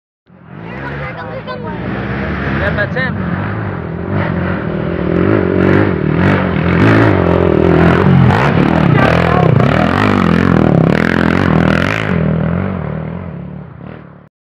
AWRCS 2011 Round 2 Wellsville Hillclimb
Short Video of me hitting the hillclimb at Wellsville.